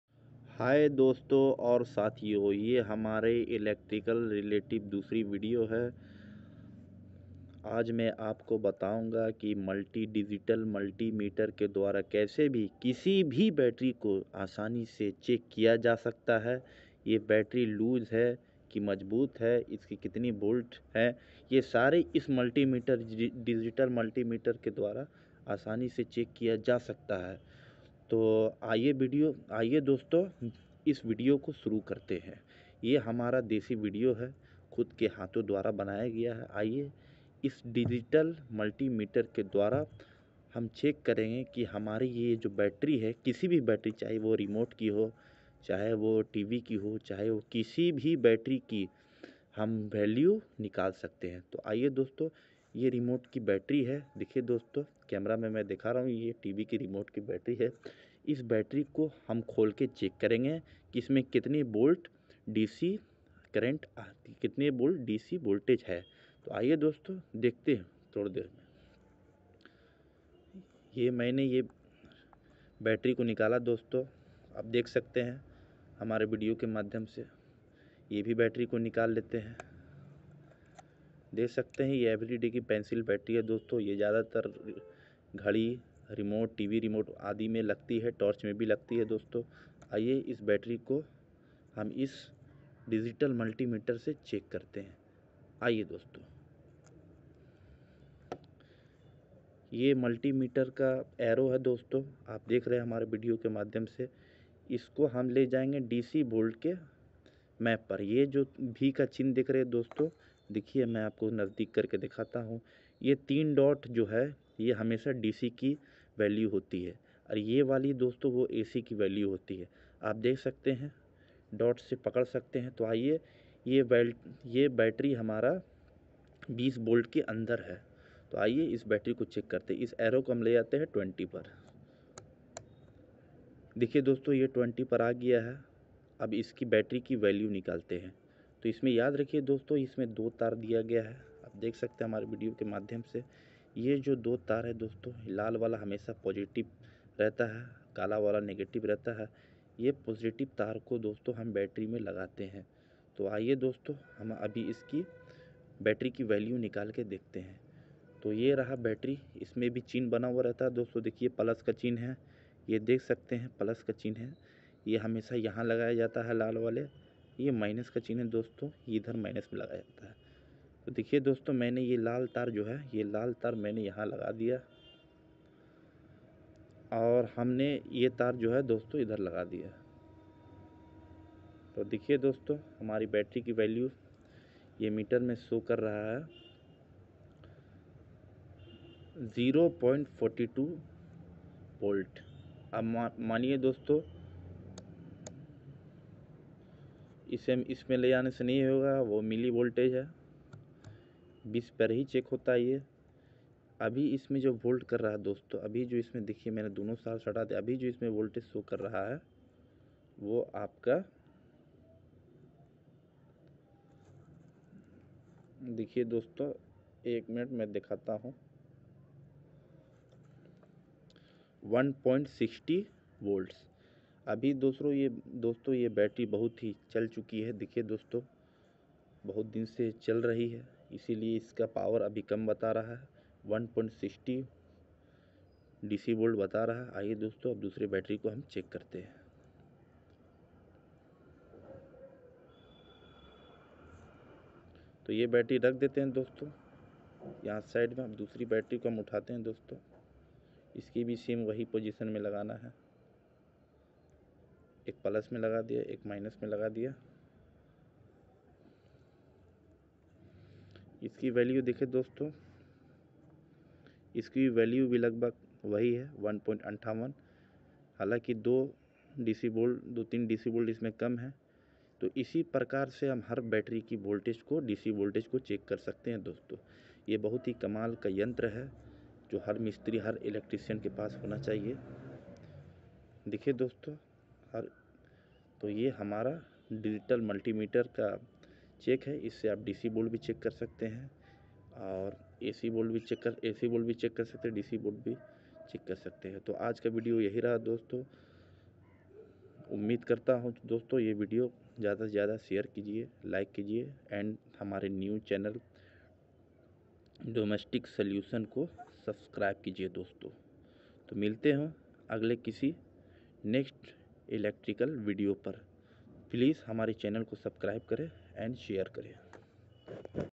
How to check battery with multimeter ! How to Check D C Battery Bolt With Multimeter.
This video explains how to bolt check DC battery with multimeter.
This is our Electrical Relative other video please like channel.